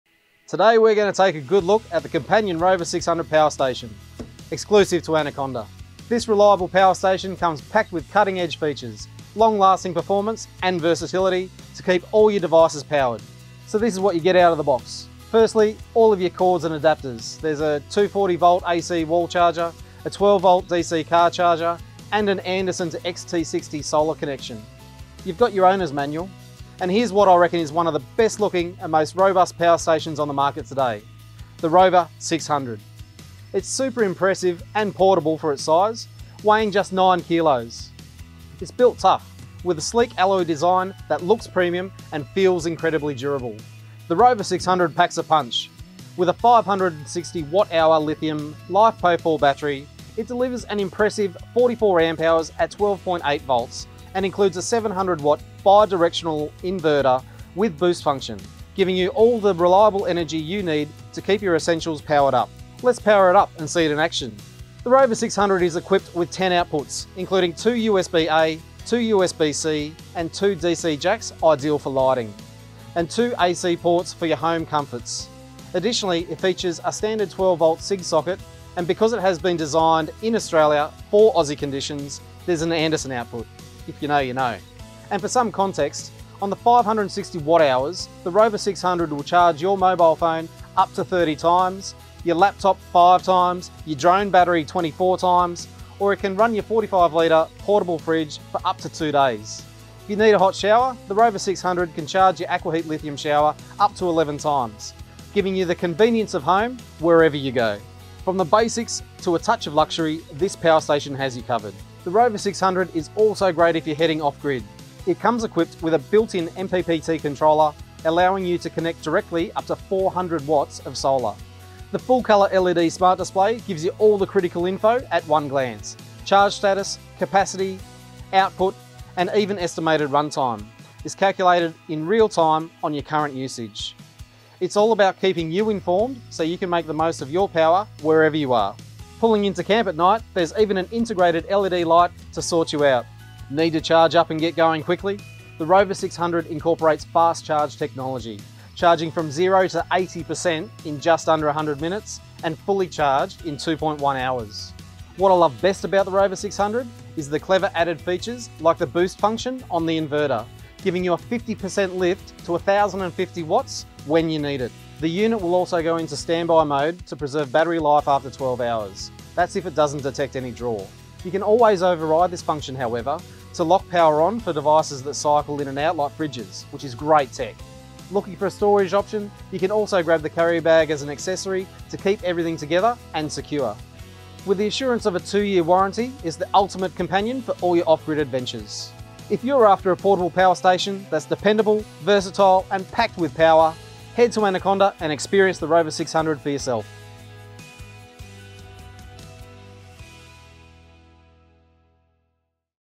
Companion Rover 600
Looking for reliable power on the go? The Rover 600 is your ultimate portable power station, perfect for camping, road trips, and off-grid adventures. With 560Wh LiFePo4 battery capacity, a 700W Pure Sine Wave inverter, and multiple output options, it keeps your essential devices running—whether it's a fridge, laptop, CPAP machine, or phone.

In this video, we’ll take you through:
✅ Unit size & portability
✅ How it works 🔌
✅ Charging options & solar compatibility ☀️

⚡ Fast charge from 0-100% in just 2 hours! ⚡

Timestamps:
00:00 – What’s Included
00:26 – Product Overview
01:04 – Ports & Outputs
01:24 – Charging
02:38 – Features

Watch now to see why the Rover 600 is a game-changer for off-grid power! 🎥🔋